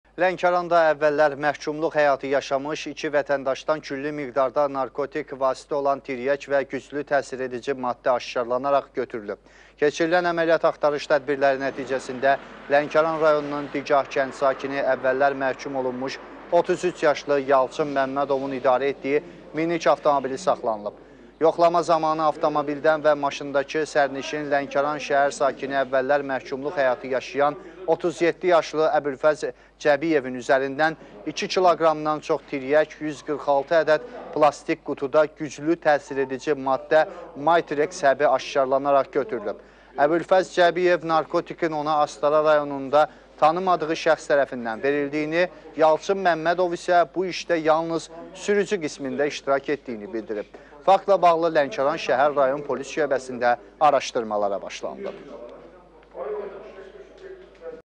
Lənkəran sakinləri külli miqdarda narkotik vasitə ilə tutuldu
Lənkəranda əvəllər məhkumluq həyatı yaşamış 2 vətəndaşdan külli miqdarda narkotik vasitə olan tiryək və güclü təsiredici maddə aşkarlanaraq götürülüb. Keçirilən əməliyyat-axtarış tədbirləri nəticəsində Lənkəran rayonunun Digah kənd sakini, əvvəllər məhkum olunmuş 33 yaşlı Yalçın Məmmədovun idarə etdiyi minik avtomobili saxlanılıb. Yoxlama zamanı avtomobildən və maşındakı sərnişin, Lənkəran şəhər sakini, əvvəllər məhkumluq həyatı yaşayan 37 yaşlı Əbülfəz Cəbiyevin üzərindən 2 kiloqramdan çox tiryək, 146 ədəd plastik qutuda güclü təsiredici maddə “Maytreks” həbi aşkarlanaraq götürülüb. Əbülfəz Cəbiyev narkotikin ona Astara rayonunda tanımadığı şəxs tərəfindən verildiyini, Yalçın Məmmədov isə bu işdə yalnız sürücü qismində iştirak etdiyini bildirib. Faktla bağlı Lənkəran Şəhər Rayon Polis Şöbəsində araşdırmalara başlanılıb.